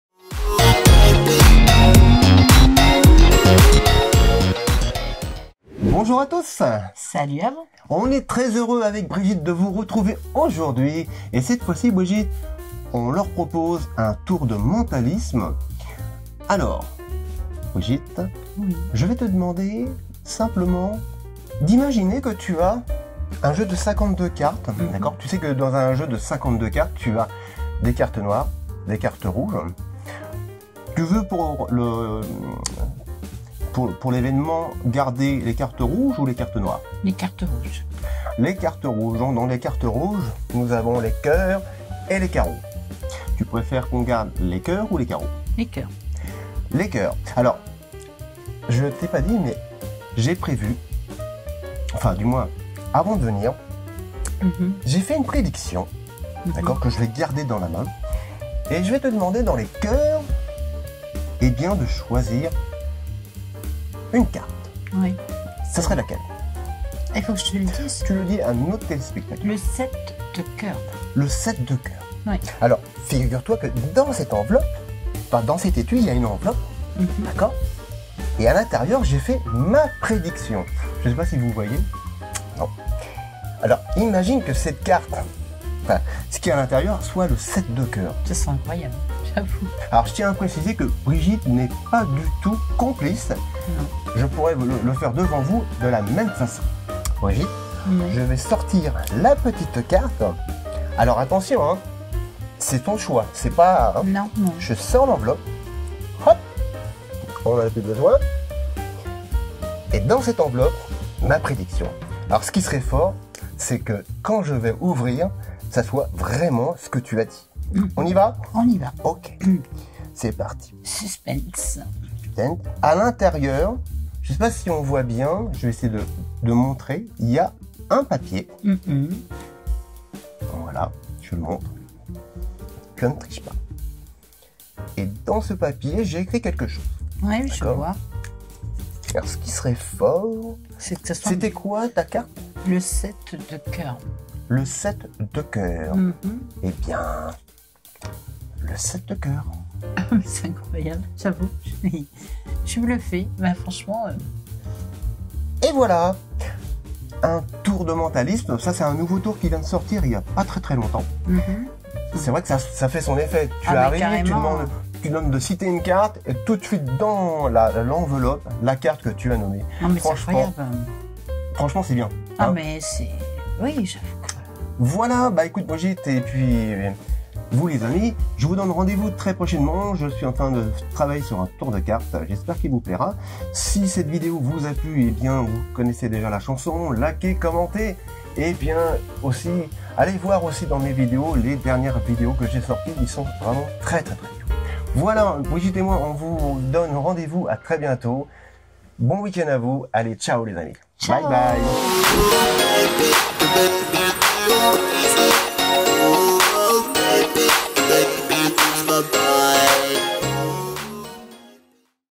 Mentalisme 👉 PRO - PHESY 👉 SMAGIC PRODUCTIONS 👉 Magic Pascal 📌 Tour de magie
👉 Tour de magie Mentalisme en vidéo sur YouTube !
Cette semaine, je vous offre en vidéo le tour de Mentalisme !
PRO - PHESY  | Tour de magie avec des cartes | Magic tricks Mentalism